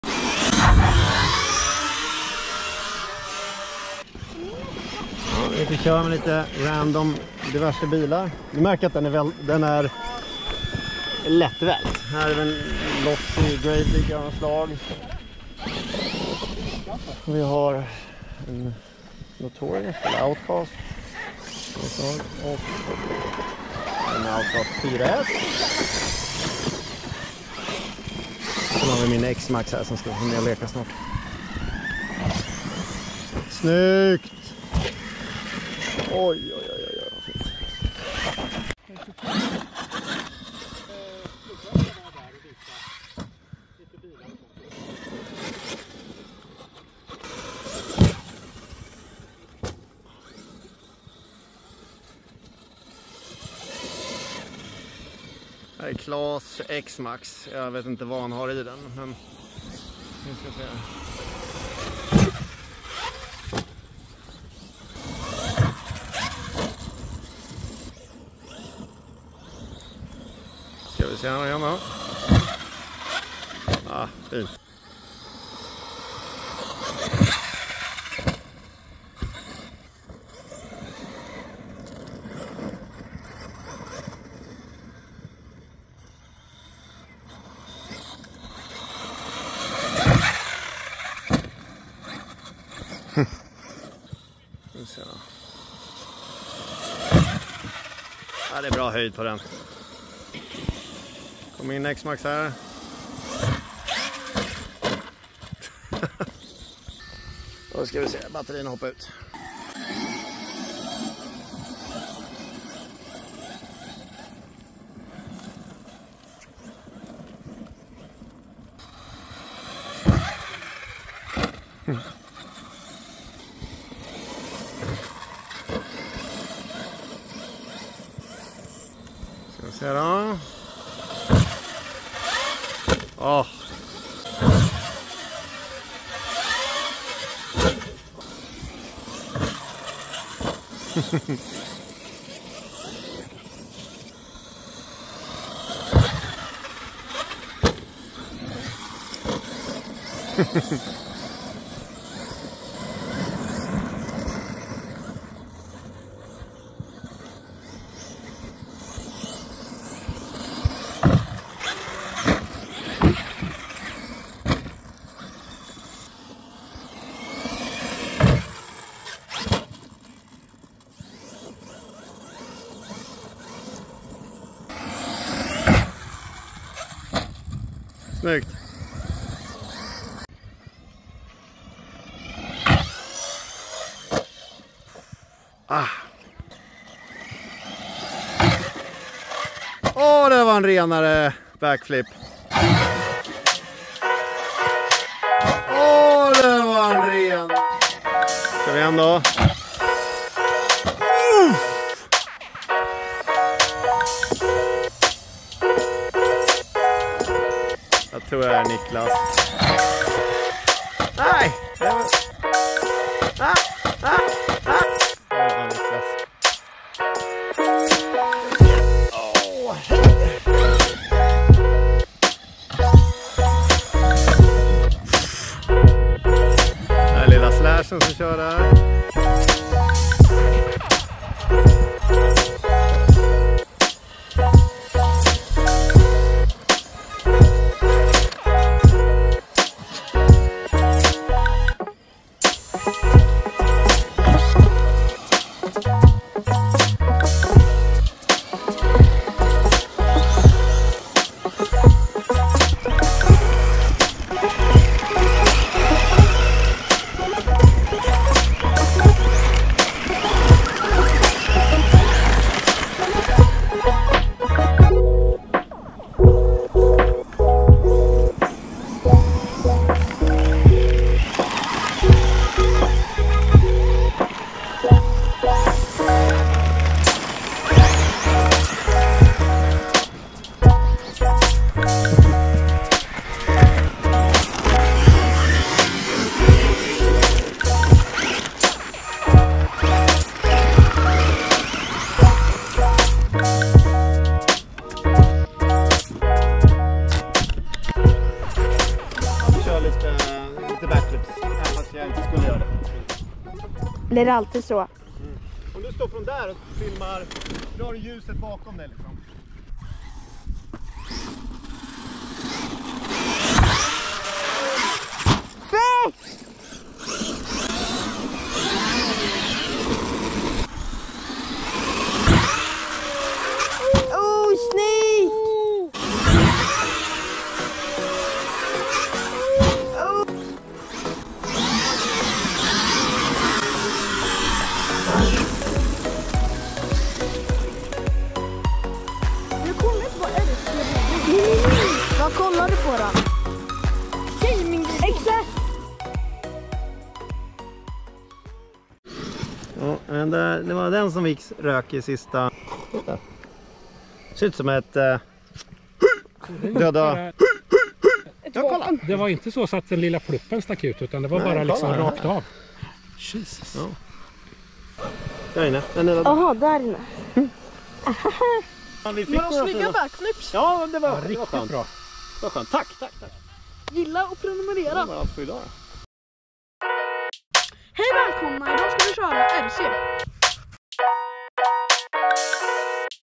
Dammiga backflips!!!
Vi dammar runt med Traxxas slash, Xmaxx, Losi LMT gravedigger och testar ramper från lekakademin.se